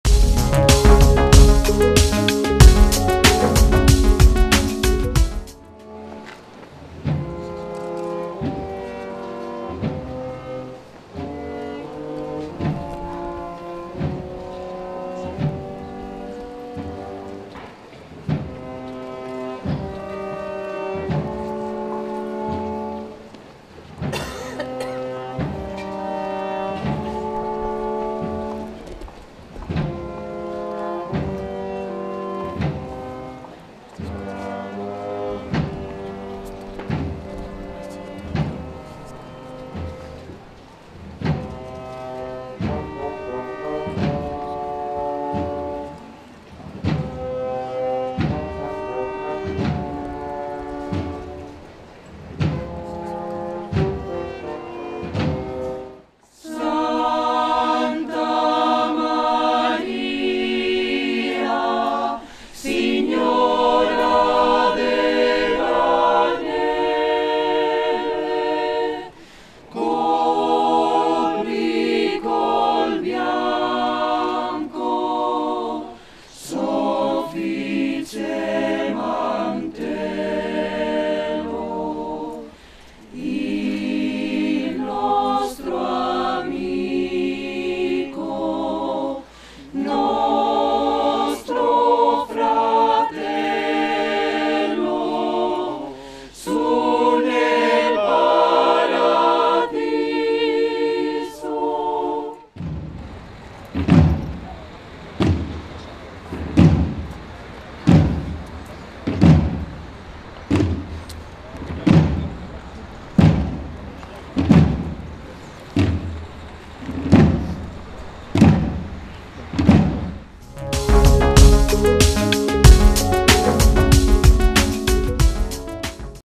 PROCESIÓN CRISTO DE LA LIBERACIÓN (SALAMANCA)
Procesión de la Hermandad del Cristo del Amor y de la Paz (Sección del Santísimo Cristo de la Liberación), dentro de la Semana Santa 2016 en Salamanca.

Imagen: atrusa producciones